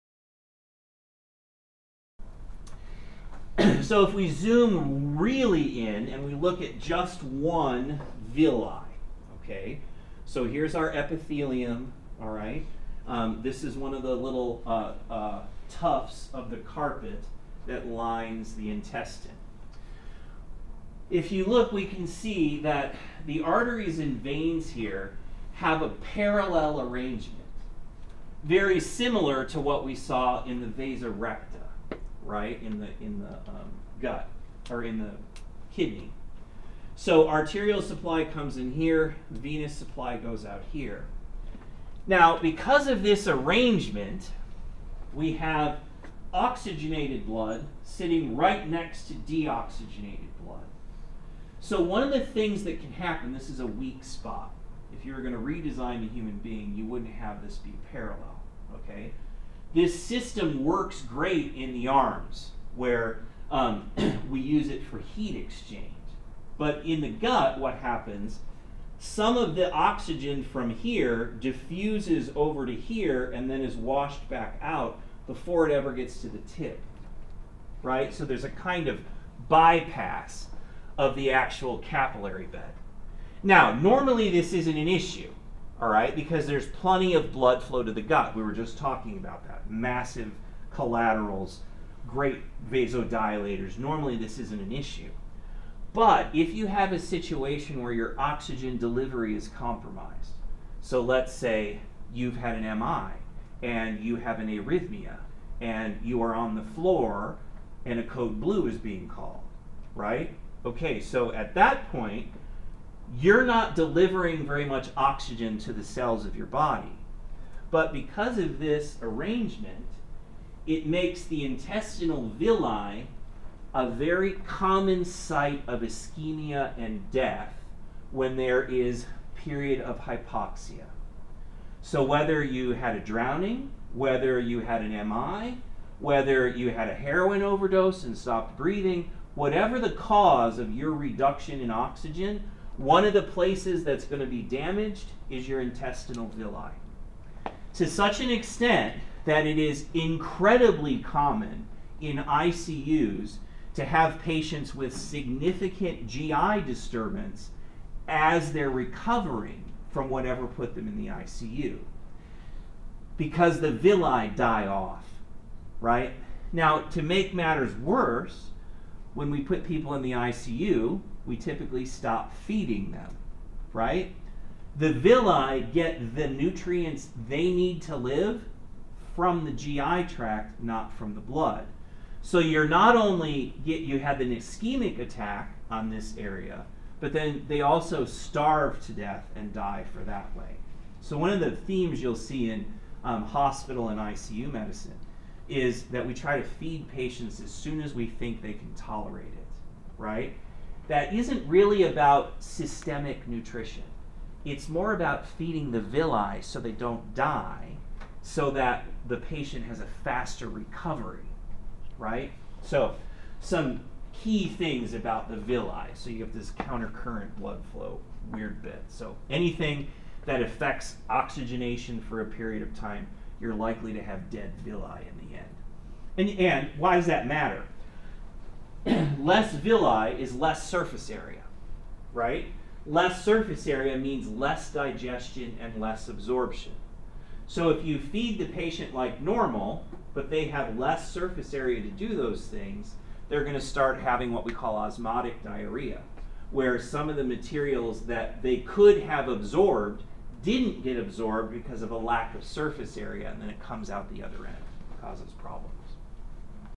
GI Physiology: Counter-current Blood Flow in Villi
The arrangement of blood vessels in the intestinal villi allows diffusion of oxygen across the villi, robbing the tips of the villi of oxygen when oxygen is low.